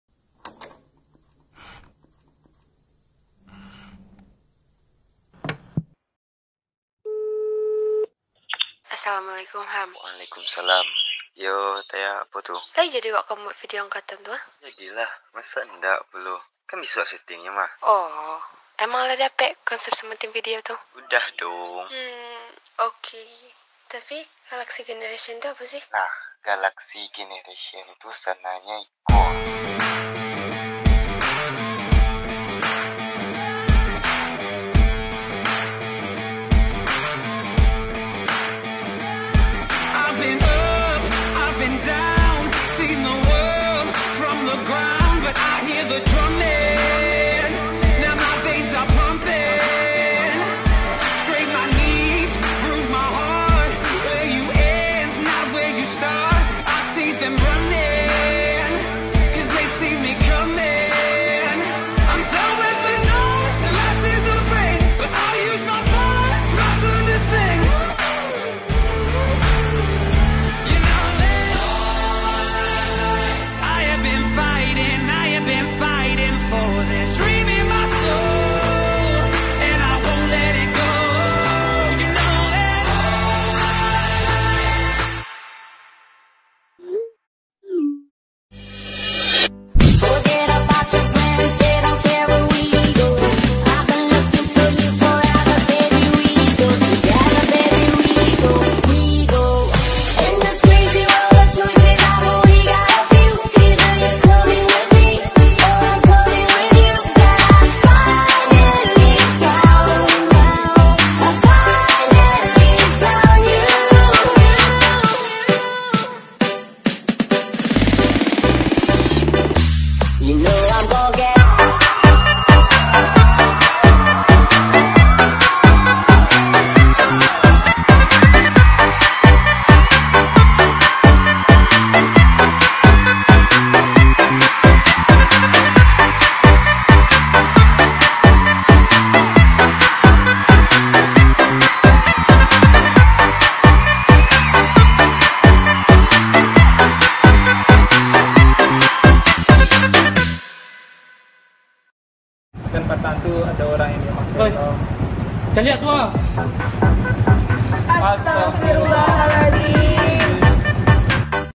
TRAILER GALAXY GENERATION -SMAN 2 LUBUK BASUNG- 2020
Terima kasih untuk 3 tahunnya teman", semuanya pasti memiliki kenang"an yang indah tersendiri pada waktunya, di mulai dari masa putih abu-abu, dan di akhiri dengan masa putih abu-abu juga, masa putih abu-abu memang banyak memiliki kenangan, dimasa remaja kita, yang lepas dari semua beban berat.
Hy teman, sekarang hanya satu pintaku, mari sukses bersama dan mari berkumpul lagi dengan kita yang sudah menjadi sukses :)
mungkin memang benar perpisahan kita ini berbeda dari yang lain, perpisahan yang tak memberikan kesan indah pada kita, tapi buat lah hari-hari yang pernah terjalin menjadi kenang"an indah, perpisahan yang sudah di rancang sedemikian rupa hanya tinggal wacana, kita usai belum pada masanya, kita istirahat belum pada waktunya, tak ada kata selamat tinggal yang kita ucapkan secara lansung {Tamat Dadakan}